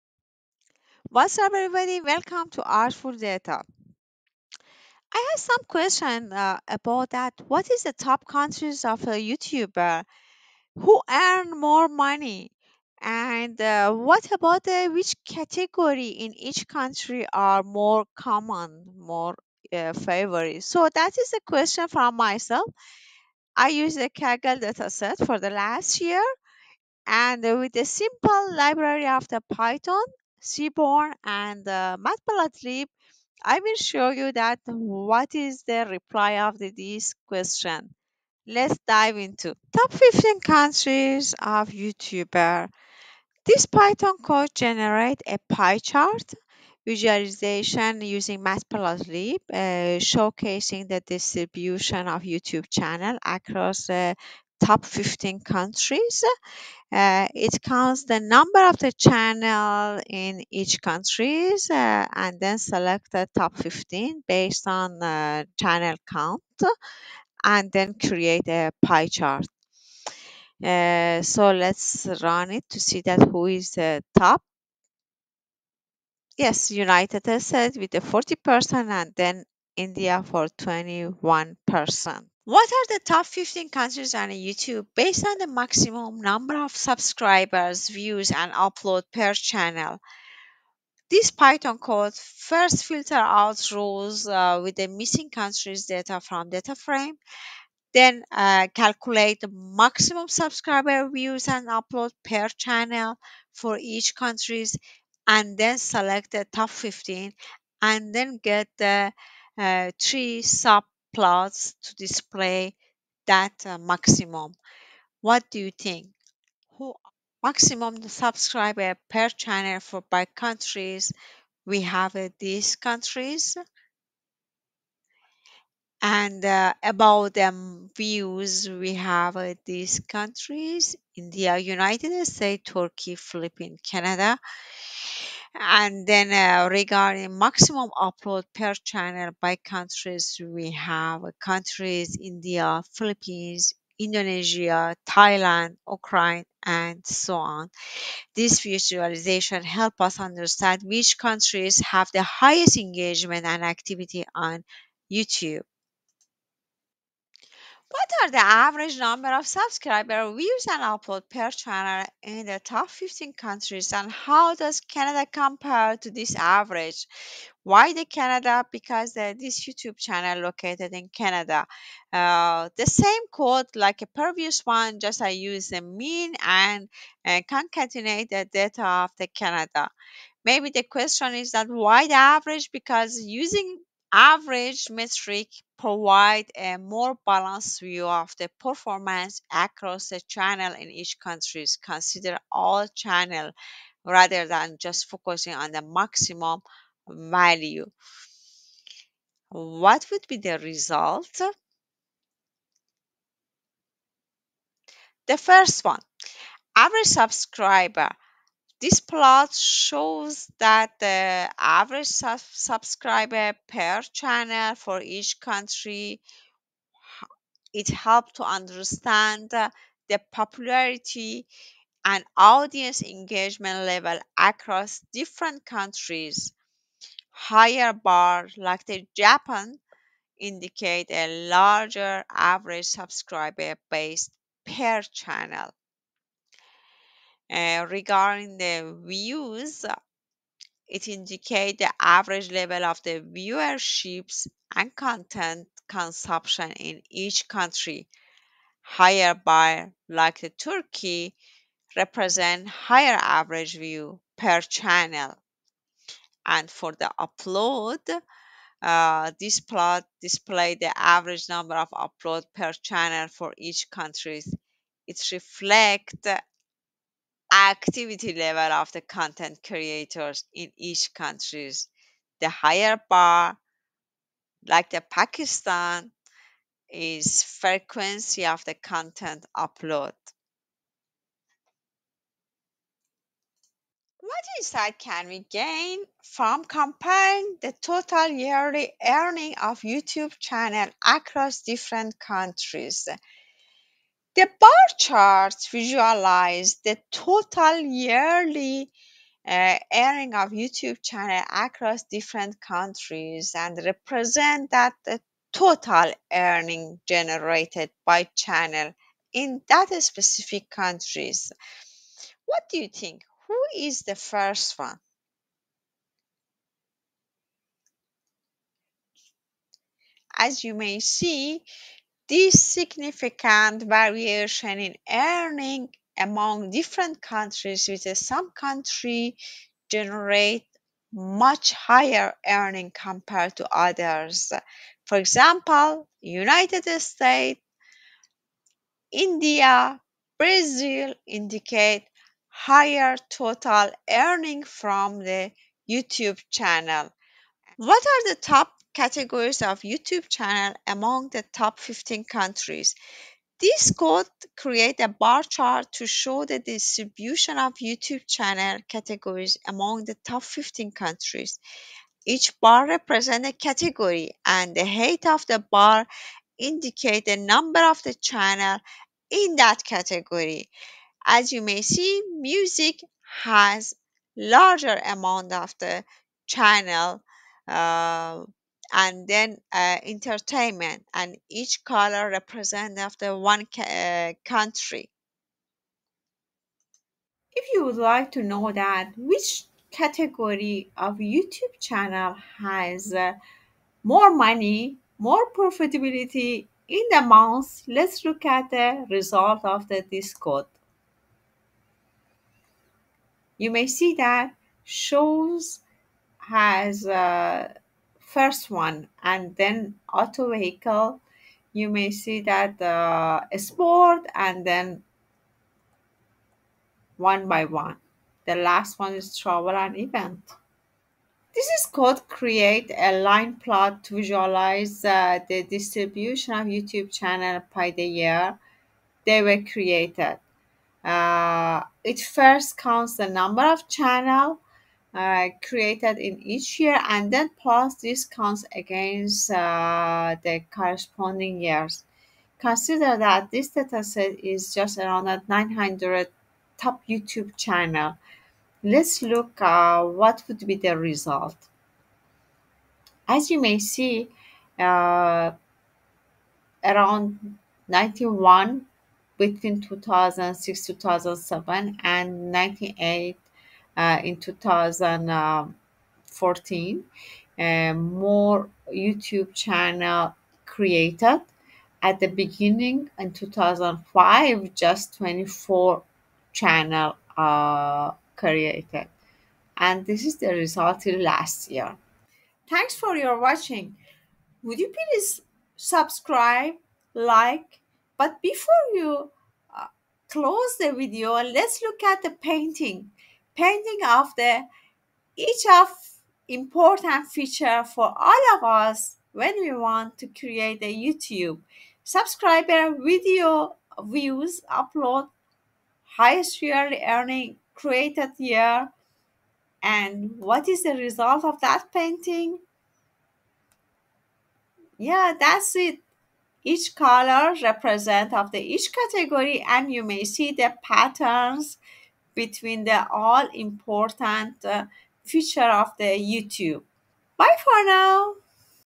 Revealing the Hidden Stats of 15 Top YouTubers
Curious about where the top YouTubers live? Find out which countries are home to the biggest YouTube stars and where they earn the most money in this video!